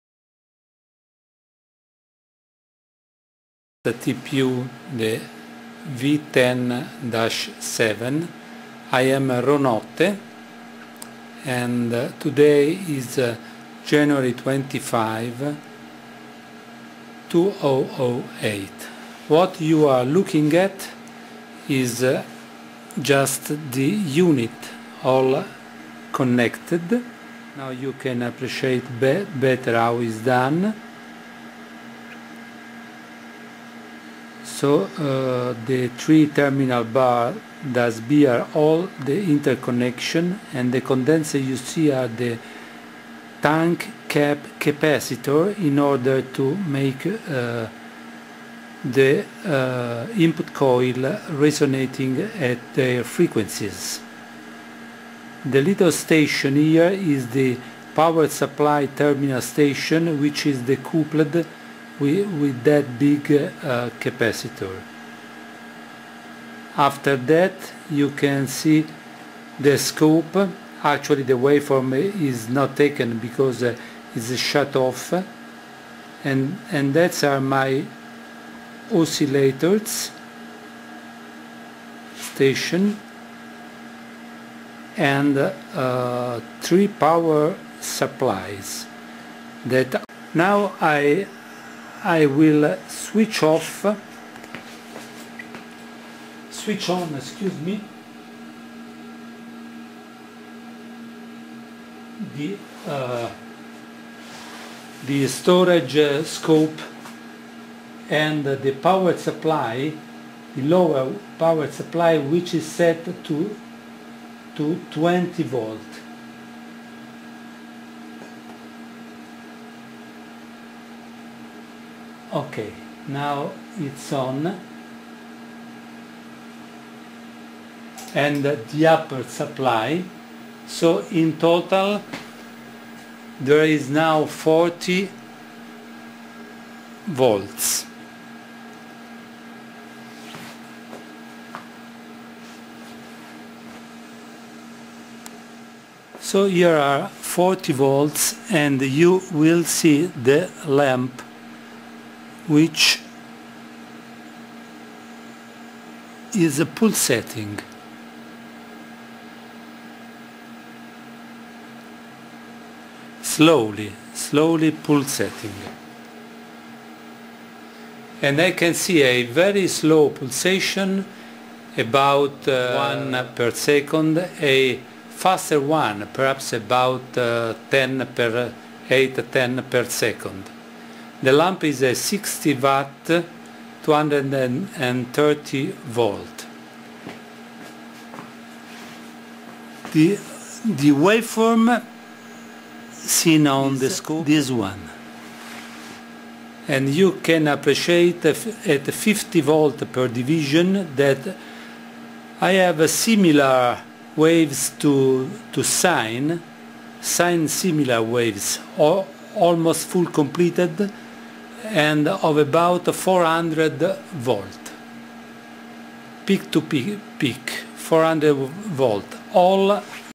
TPU V10-7
This is a 3Stack TPU done accordingly dfro suggestions and some other improvements. I'm using 3 frequencies in the form of 2 microseconds pulses with 40V amplitude as input.